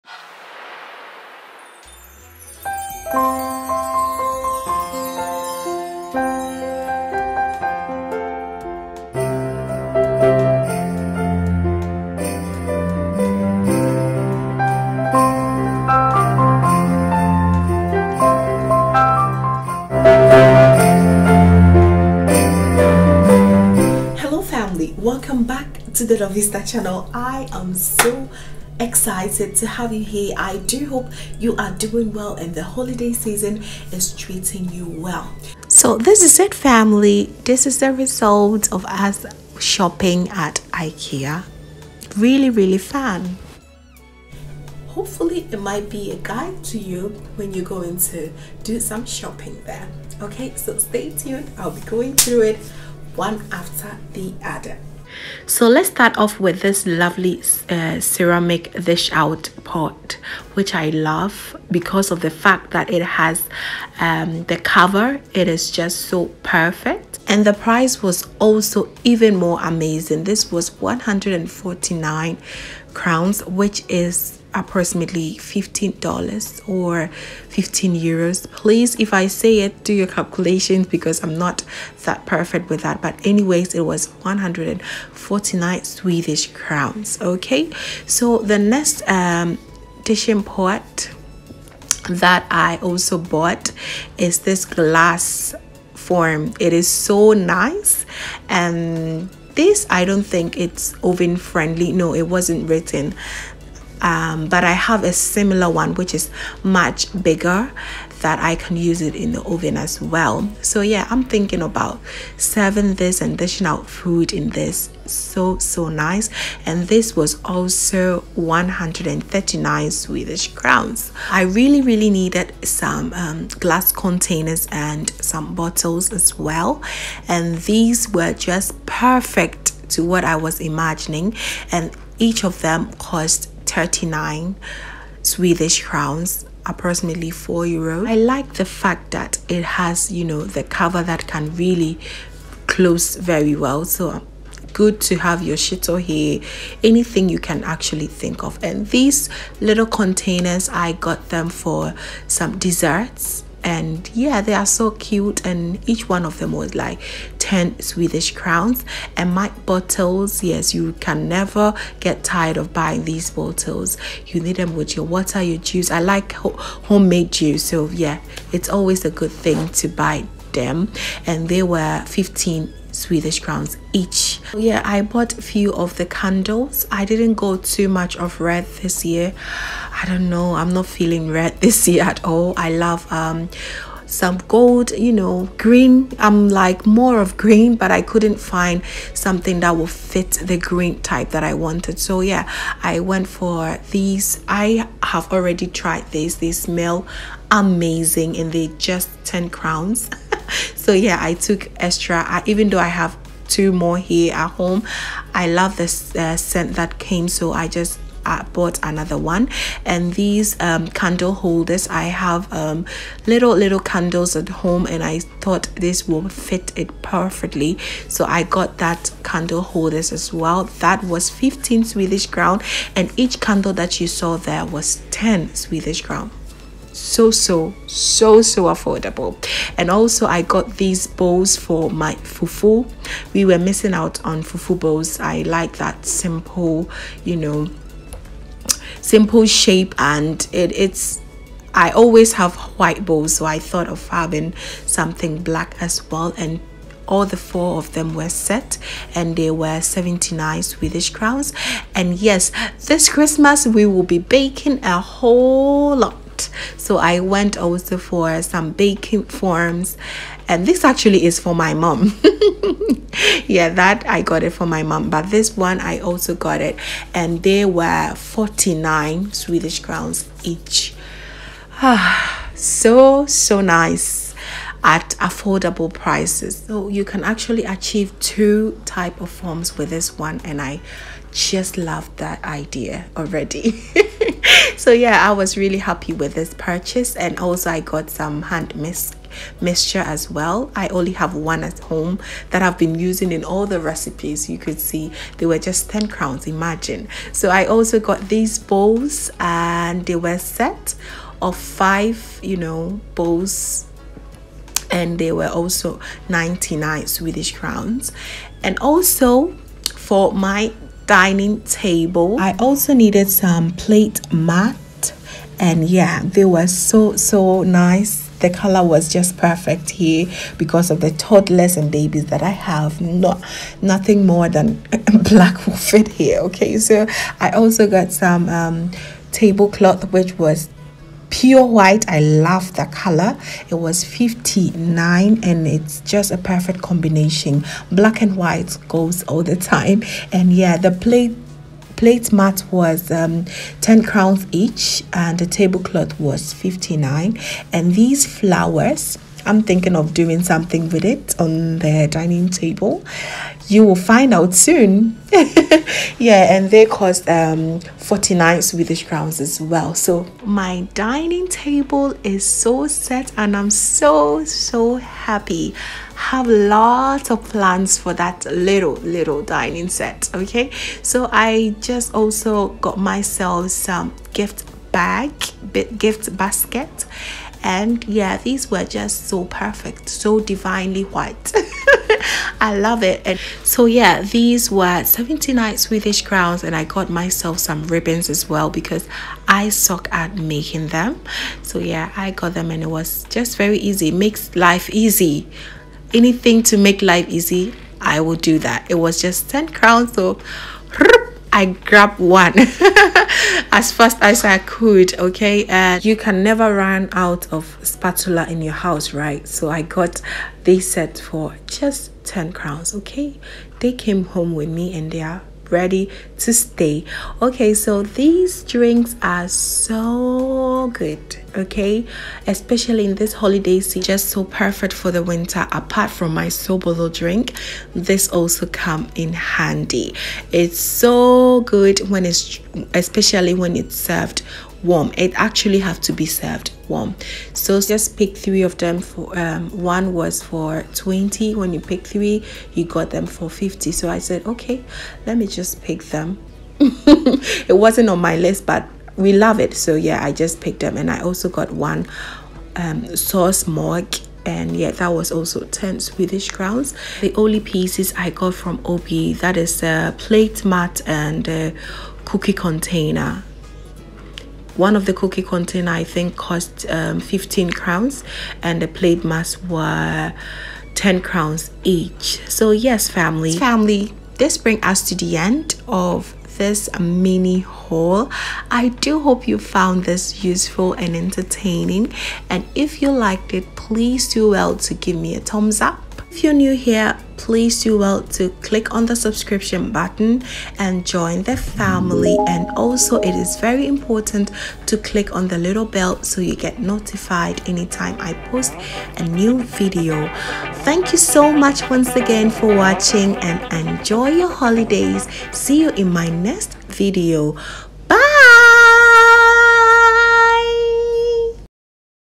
MY IKEA KITCHENWARE HAUL HOLIDAYSEASON VLOGMAS FOODMAS IKEASWEDEN.
HELLO LOVELY FAMILY I TRUST YOU ARE DOING WELL AS PROMISED IN MY PREVIOUS VIDEO THIS IS THE KITCHEN WARE HAUL I GOT FROM IKEA AND OOB I DO HOPE YOU FIND IT USEFUL THIS ITEMS WILL DEFINITELY  HELP YOUR IN  YOUR KITCHEN . CONTINUE TO ENJOY YOUR HOLIDAYS.

I DO NOT OWN COPY WRITES OF THE BACKGROUND TRACK,

WATCH THE RELATED VIDEO HERE